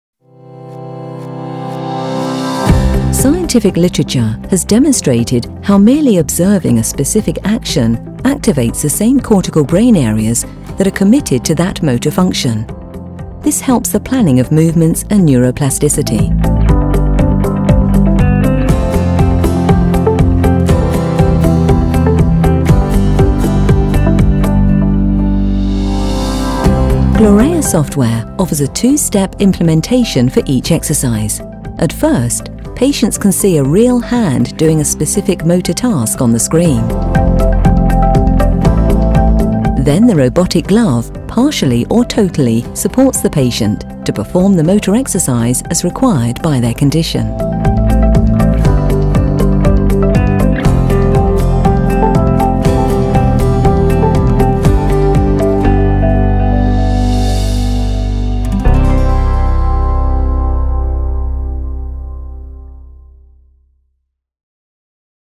Gloreha Sinfonia: upper limb rehabilitation - Action Observation Therapy
Action-observation therapy is based on scientific evidence. Observation of an active action stimulates the same cortical brain areas committed to motor functions. Gloreha Sinfonia includ action observation therapy and each exercises can be set in two steps: observation and execution.